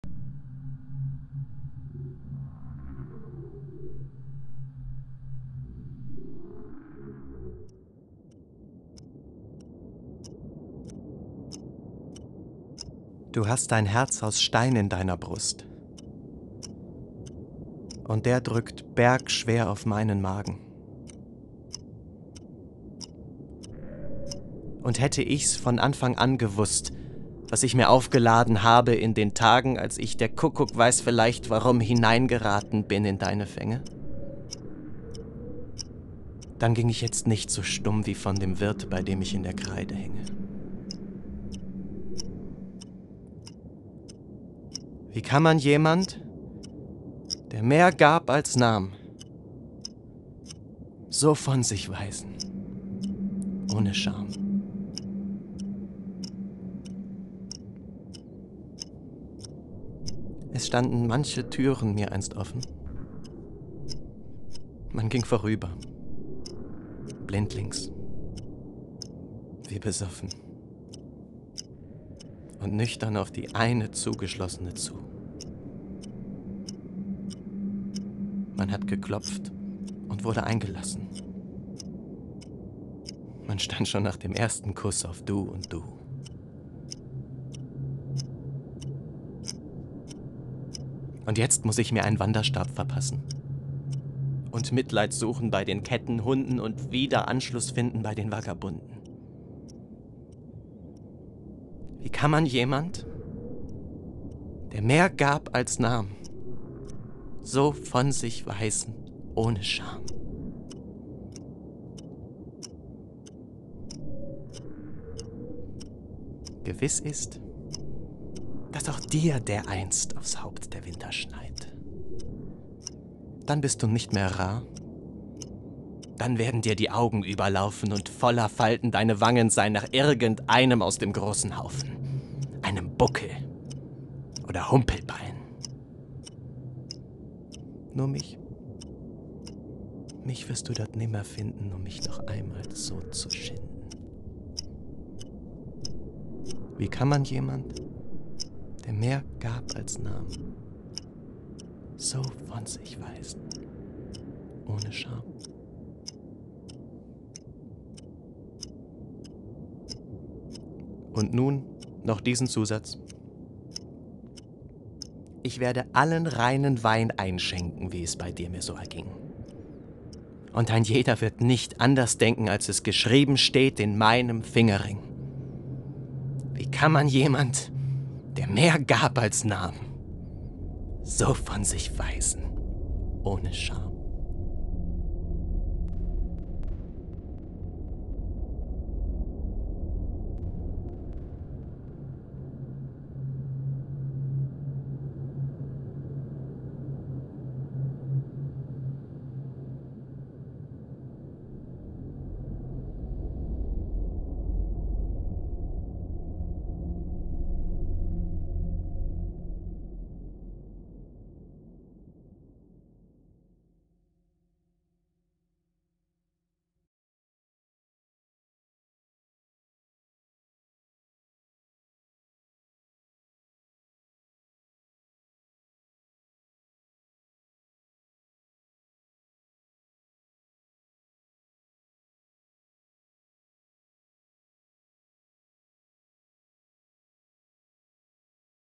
Melancholieverstand
"Isn't it conceivable that some people do suffer from a soul-driven substance that brings about their downfall?" -- these words by Honoré de Balzac built the fundament of a poetry based video art series about human isolation and loneliness.